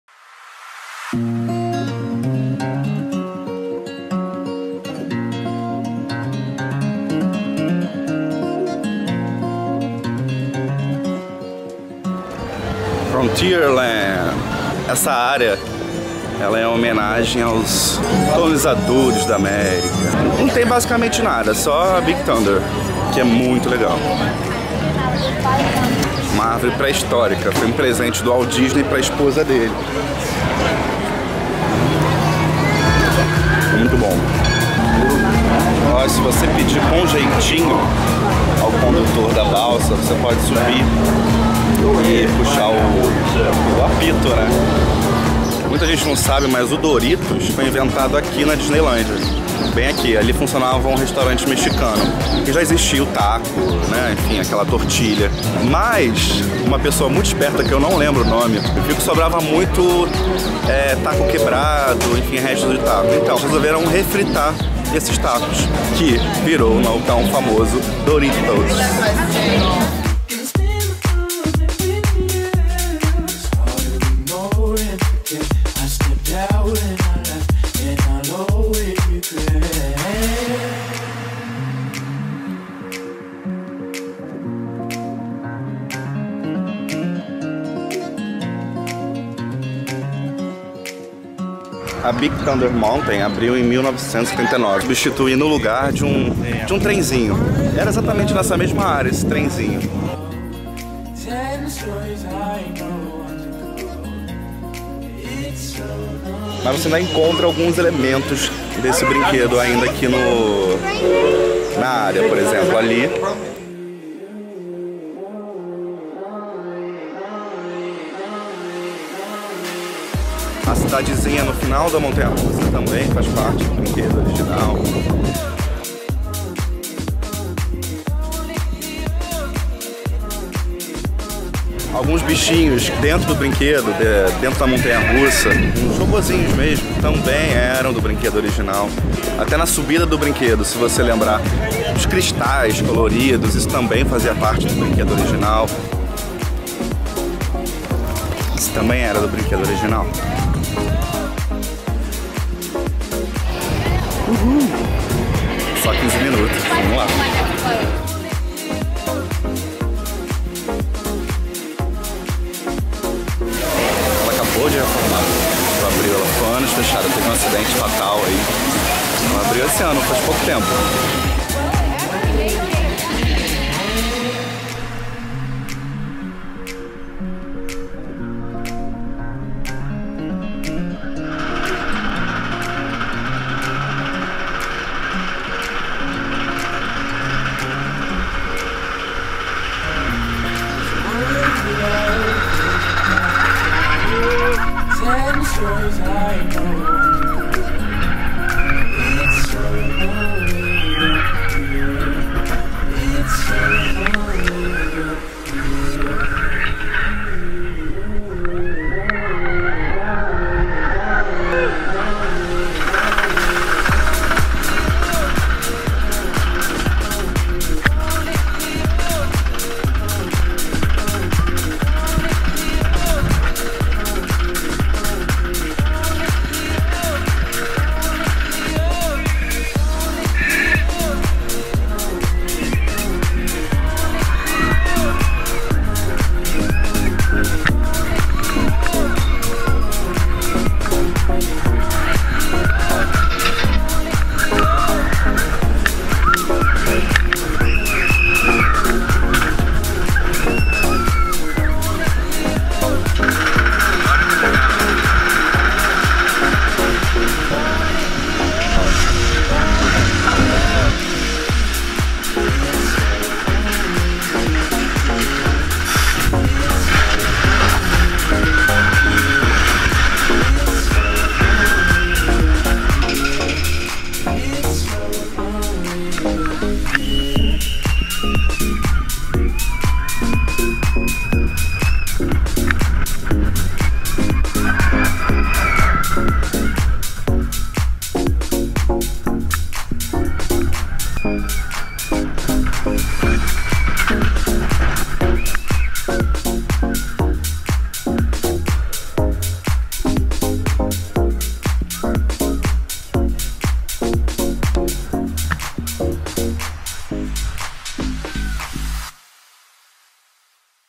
ParquesPeloMundo@Disneyland_P3
Um passeio e curiosidades na Frontierland, Disneyland